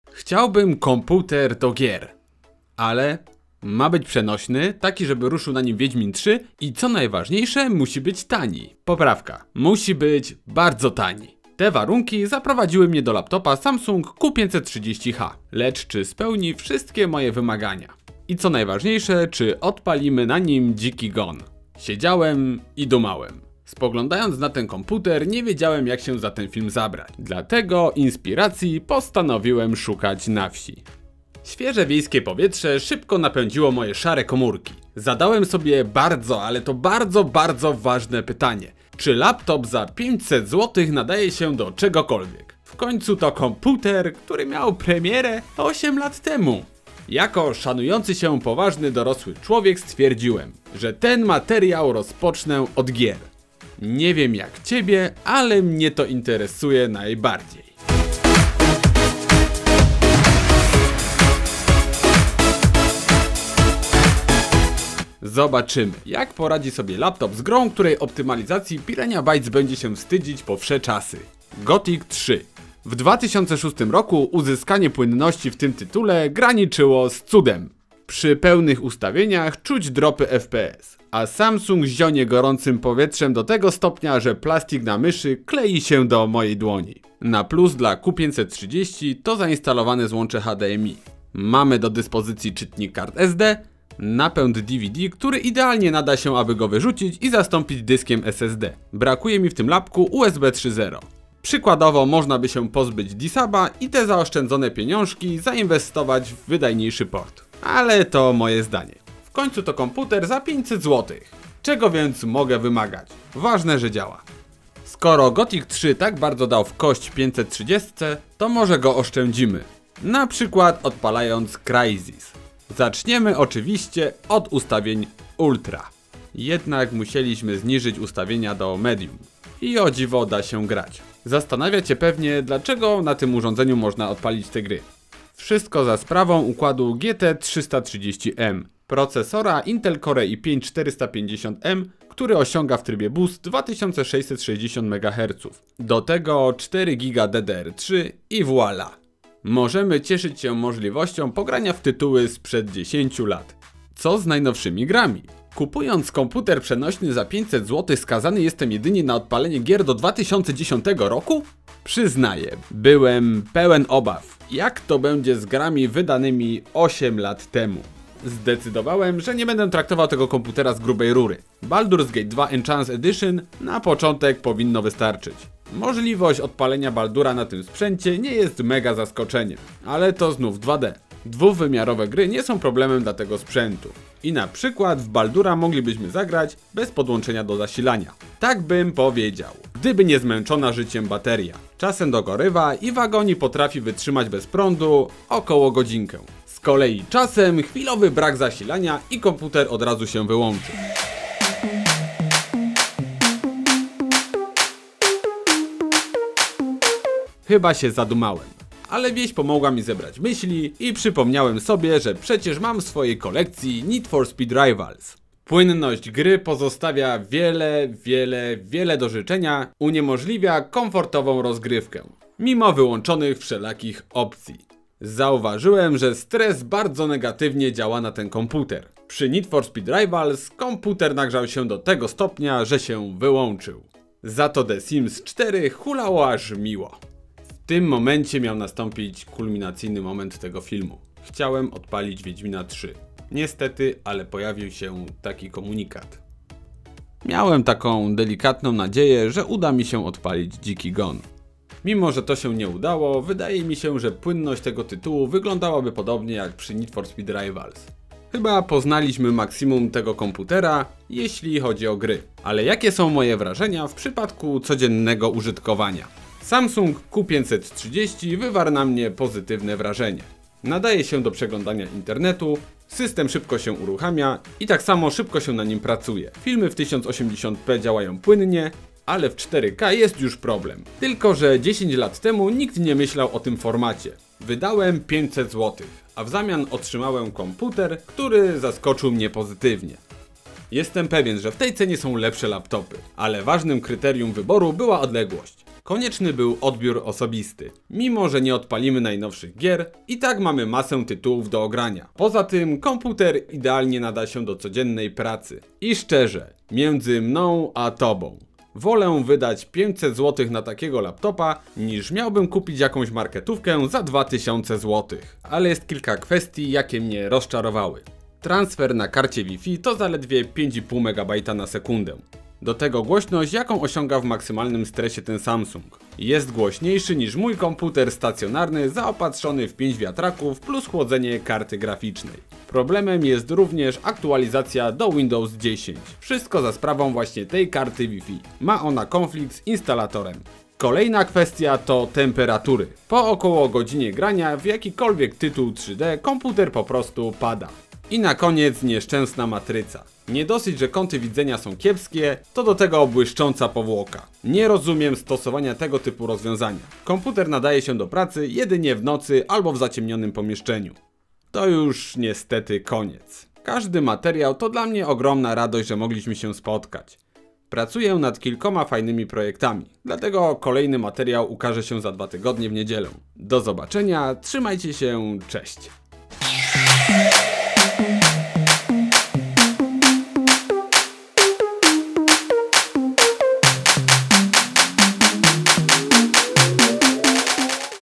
Czy warto kupić używanego Laptopa do gier za 500 złotych? Może lepiej wybrać nowego z Marketu?
Wieś jest jednym z najlepszych miejsc by znaleźć inspirację. Jak w tytule, czy warto za 500 złotych kupić laptopa żeby pograć, z jednej strony wydawać się może że taki stary komputer to już złom, ale z drugiej strony czy proces technologiczny tak się rozwinął żeby te sprzęt niedomagał. Nie pozostaje nic innego jak obejrzeć ten materiał i przekonać się samemu.

Widziałeś film Game Pass to najlepsza usługa chmurowa?